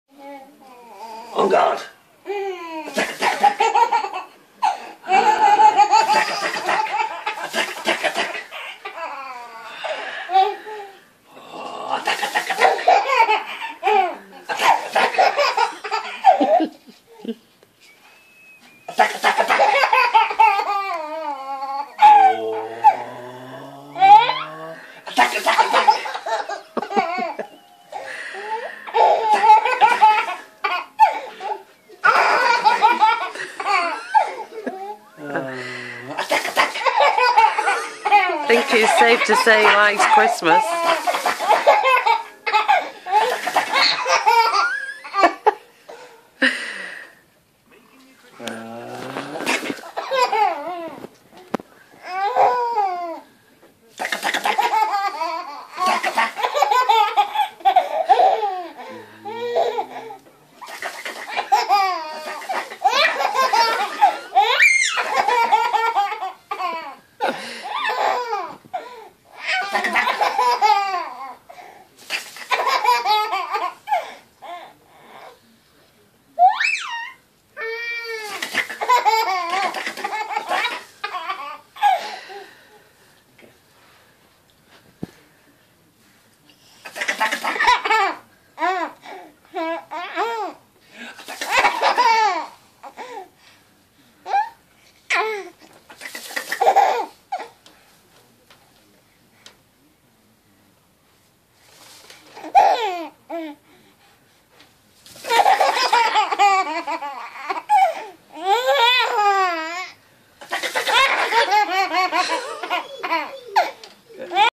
Baby laugh attack, Christmas tree
Our little man laughing his head off, while being 'attacked' by the top of the Christmas tree!